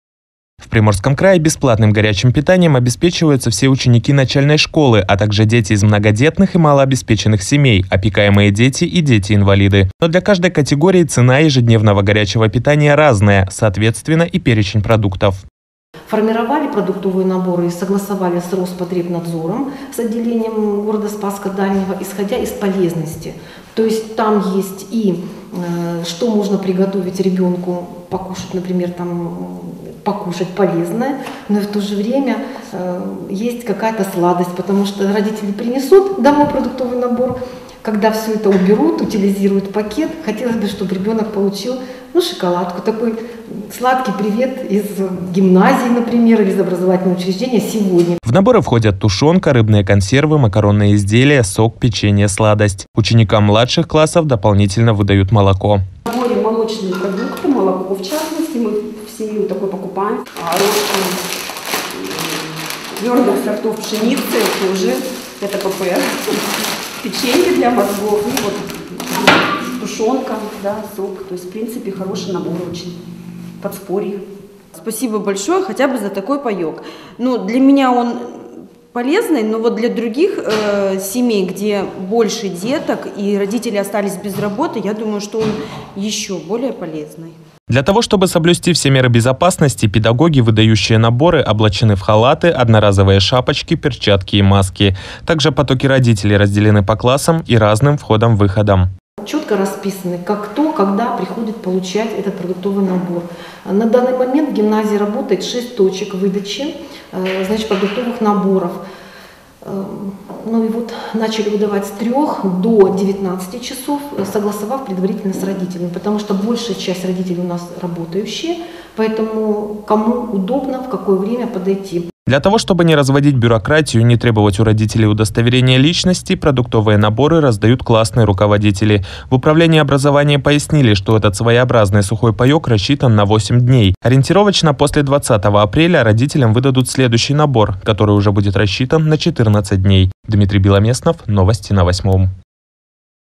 Питание детей
В Приморском крае бесплатным горячим питанием обеспечиваются все ученики начальной школы, а также дети из многодетных и малообеспеченных семей, опекаемые дети и дети-инвалиды. Но для каждой категории детей цена ежедневного горячего питания разная, соответственно и перечень продуктов разный.